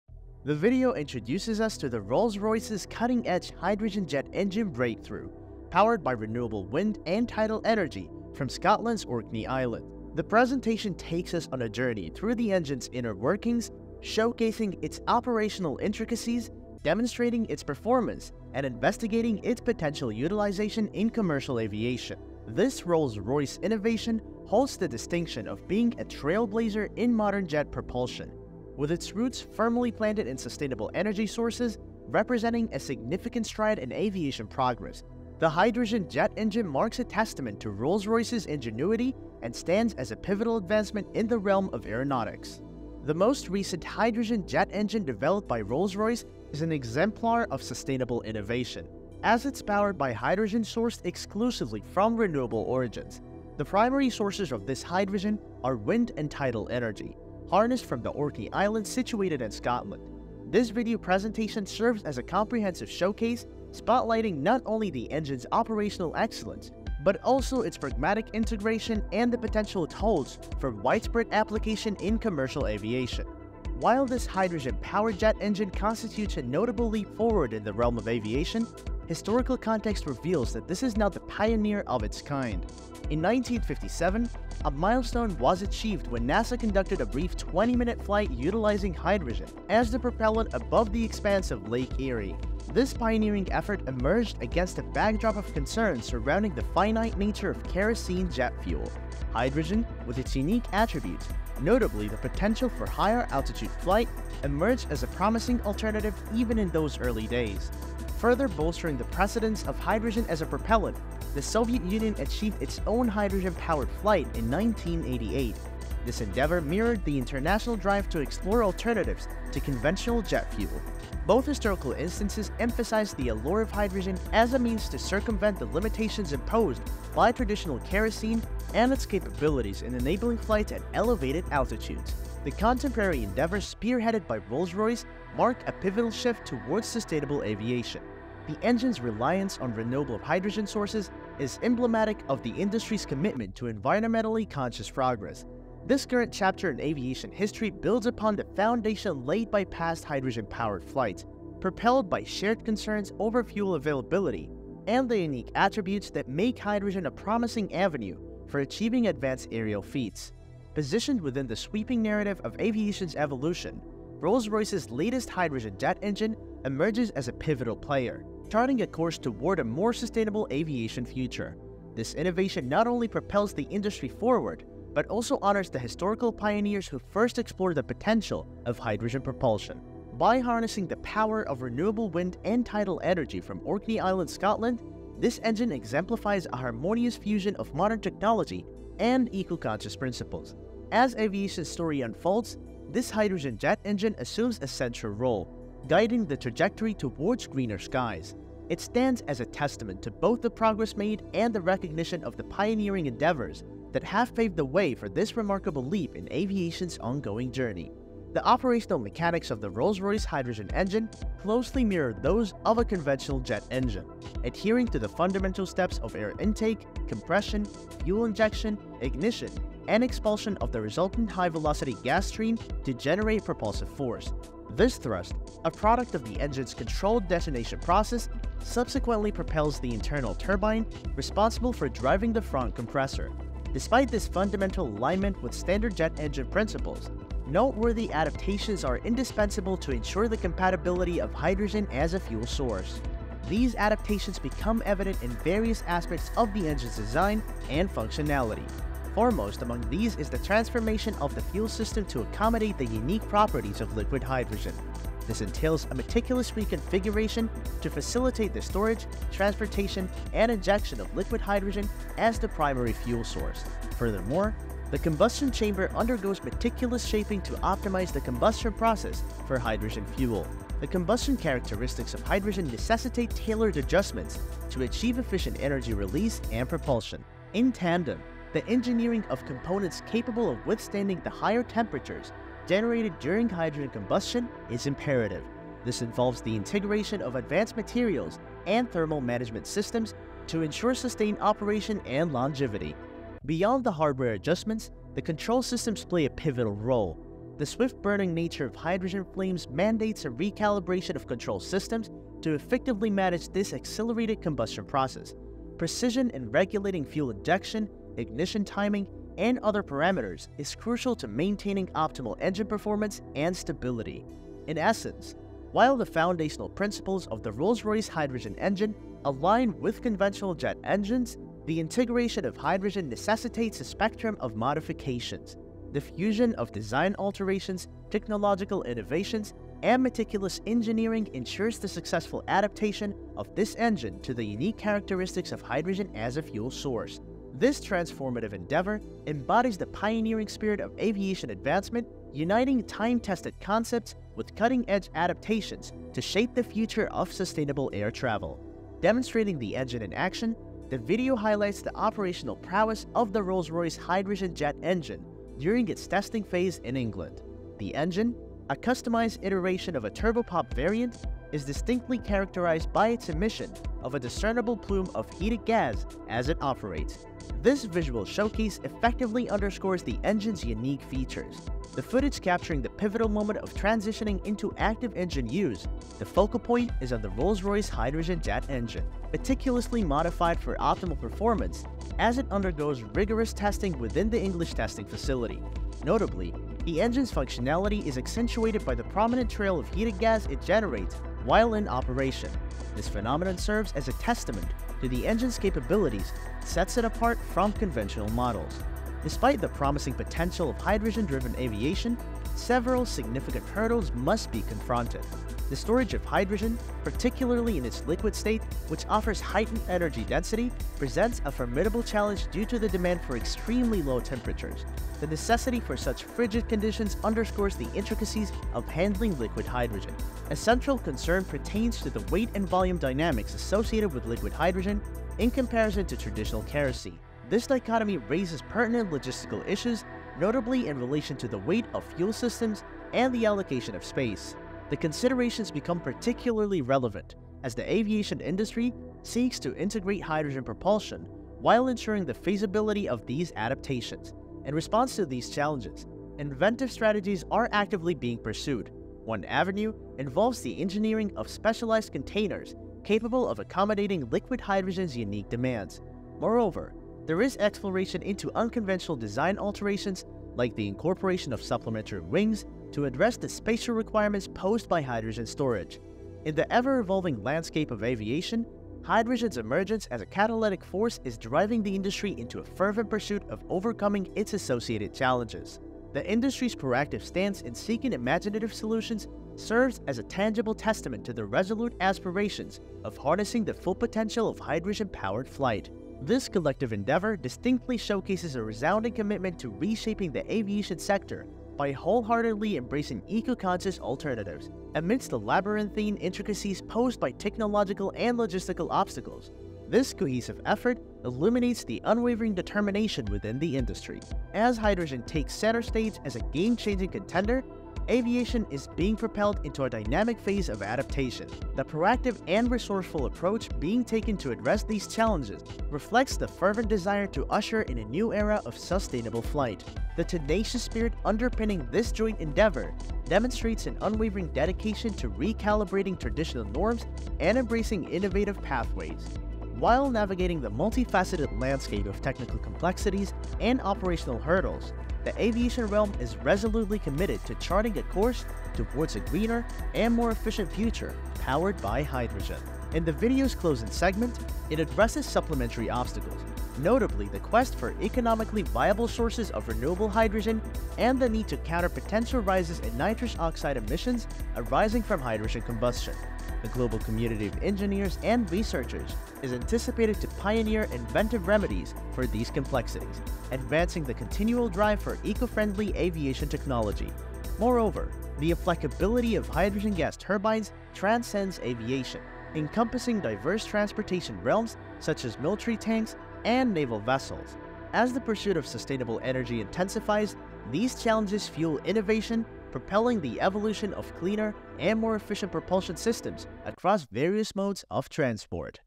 The Most Powerful Hydrogen JET ENGINE From Rolls Royce Change the WHOLE Aviation Industry! SHOCKING!
The video introduces us to Rolls Royce's cutting-edge hydrogen jet engine breakthrough, powered by renewable wind and tidal energy from Scotland's Orkney Islands. The presentation takes us on a journey through the engine's inner workings, showcasing its operational intricacies, demonstrating its performance, and investigating its potential utilization in commercial aviation. This Rolls Royce innovation holds the distinction of being a trailblazer in modern jet propulsion, with its roots firmly planted in sustainable energy sources. Representing a significant stride in aviation progress, the hydrogen jet engine marks a testament to Rolls Royce's ingenuity and stands as a pivotal advancement in the realm of aeronautics.